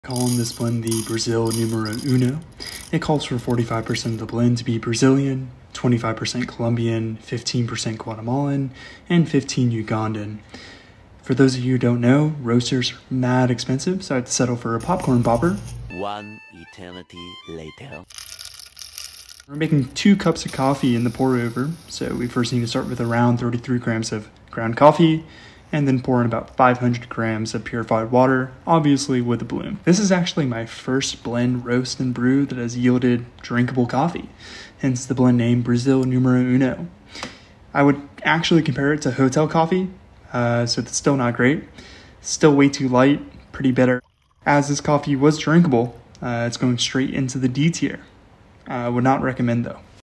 Brazil Numero Uno Coffee Blend | Coffee Kink
This coffee blend calls for
- 45 % Brazilian Single Origin Beans
- 25 % Colombian Single Origin Beans
- 15 % Guatemalan Single Origin Beans
- 15 % Ugandad Single Origin Beans